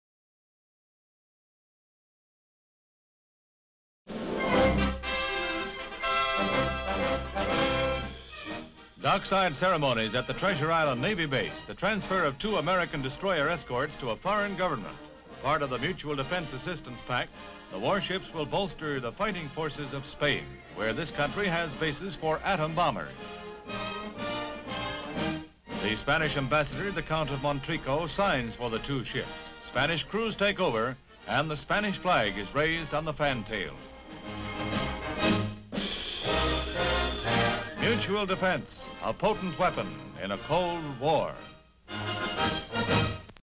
Transfer of two American destroyers to Spain under Mutual Defense Assistance Pact...HD Stock Footage
Transfer of two American destroyers to Spain under Mutual Defense Assistance Pact.

Spanish crew lined up at San Francisco's Treasure Island Navy Base. Two American destroyers transferred to Spain. Naval officers and Spanish Ambassador arrive. Spanish ambassador signs for two destroyers under Mutual Defense Assistance Pact. Naval officers salute the destroyers leaving the docks. Location: San Francisco California. Date: May 16, 1957.

57,000+ broadcast-quality historic clips for immediate download.